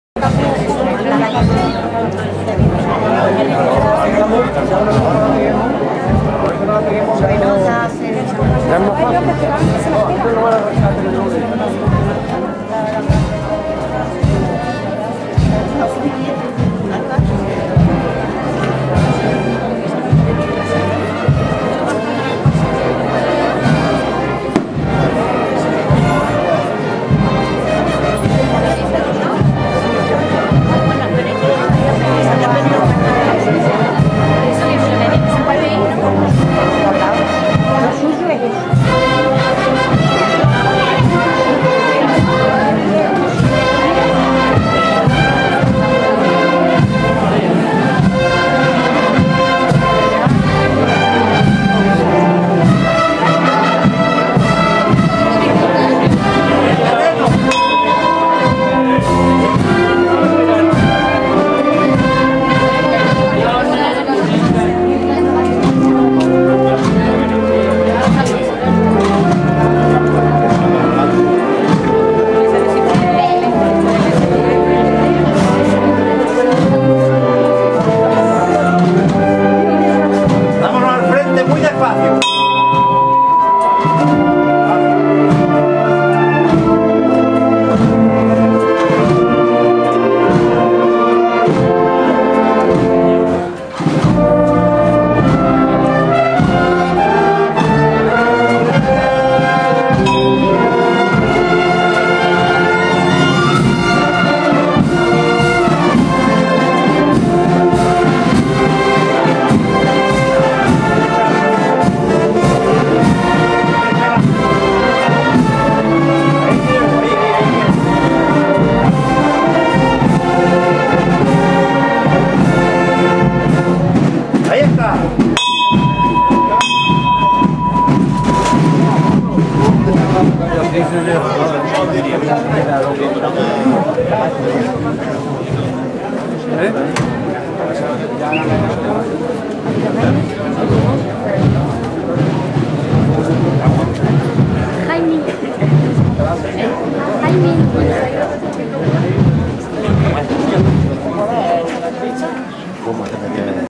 Virgen de Loreto y San Roque en el 175 Aniversario(V). Dos Torres(Córdoba)
El 14 de septiembre, ha sido un día Historico para Dos Torres...En la Celebración del 175 Aniversario de la Unión de las dos Villas, Torremilano y Torrefranca, en el actual Dos Torres... Han procesionado juntos los dos Patronos, la Virgen de Loreto y San Roque...Se le hara un Quinario... y volverán a Procesionar juntos el domingo 21 de septiembre del 2014.